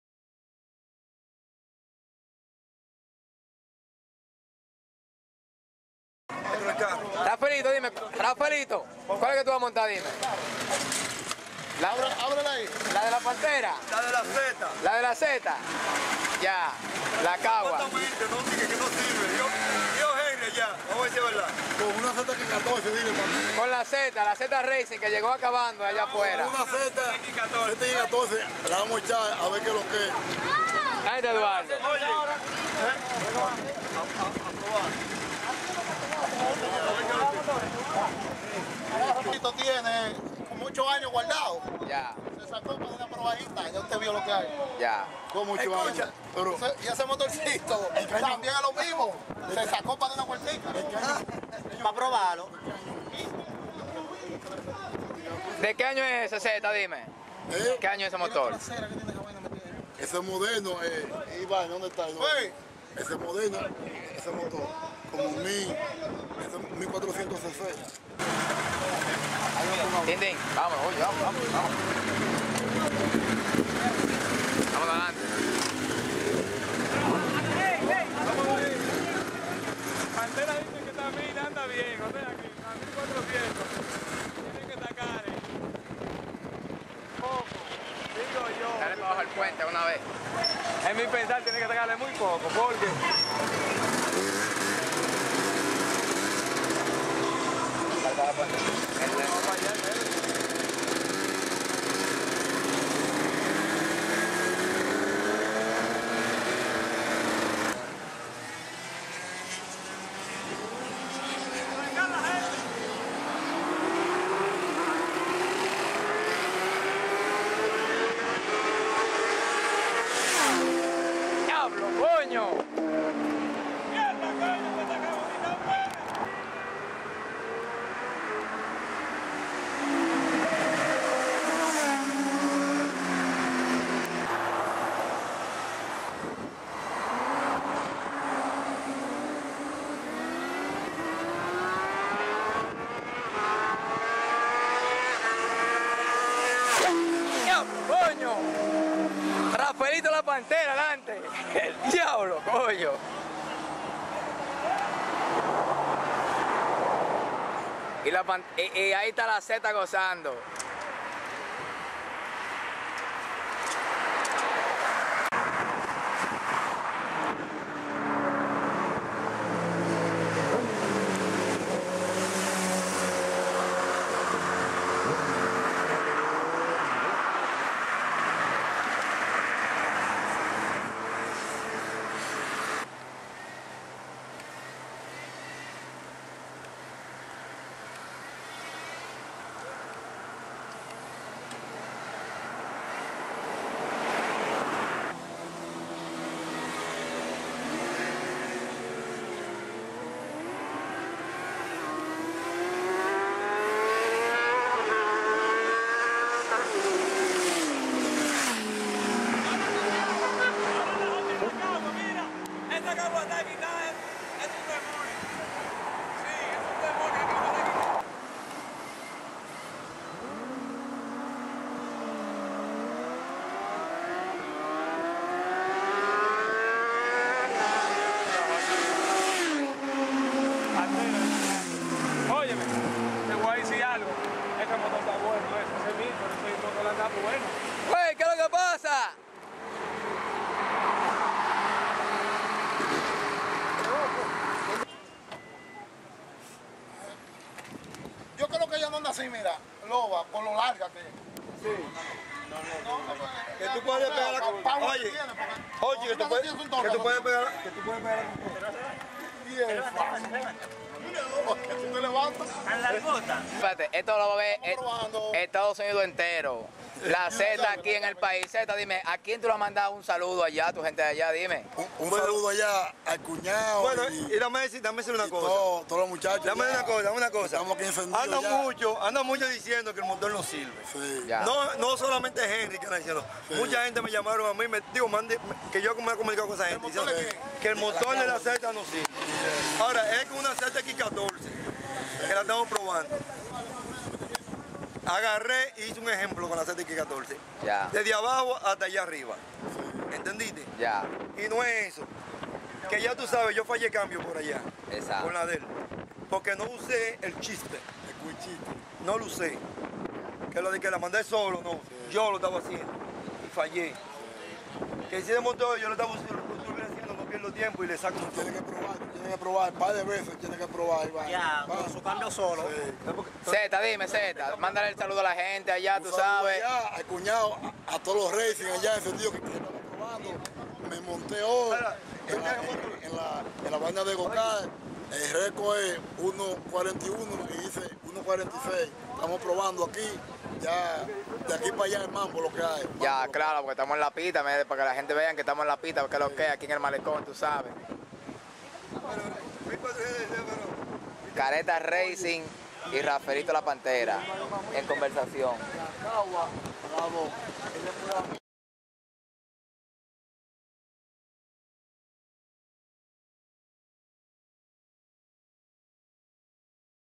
Jochy mamola y Rafelito la Pantera probando kawasaki 1000 vs kawasak zx14
Rafelito la pantera  con la kawasaki  1000 de la Z
y Jochy mamola en la  kawasak zx14
probando en el malecon
aunke despues intercambian motos.